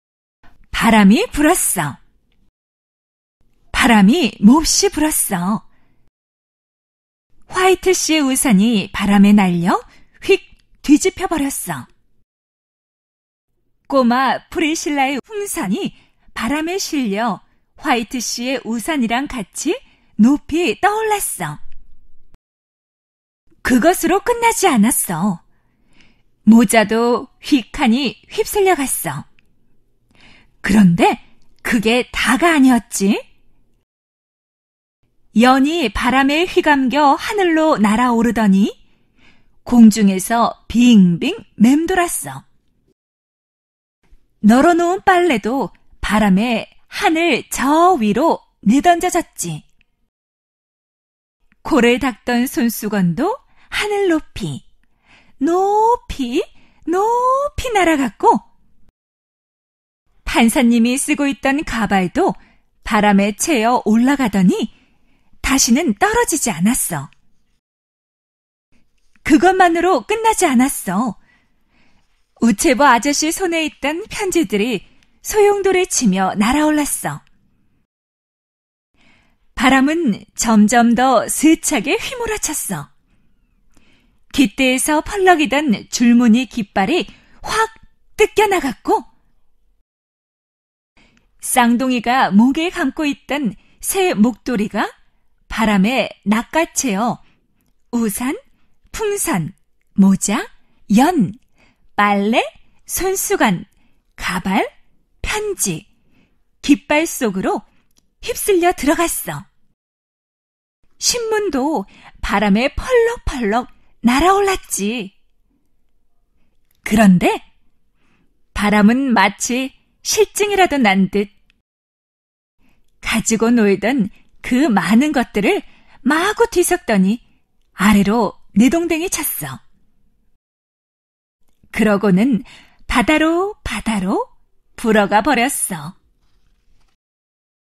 유아 │동화 │바람이 불었어 │플래시 │애니메이션 │딩동키즈 어린이 동화
- 유치원 유아들의 교육을 목적으로 만든 애니메이션

- 교육용으로 만든 자료입니다. 선생님들은 수업시간에 할 수 있습니다.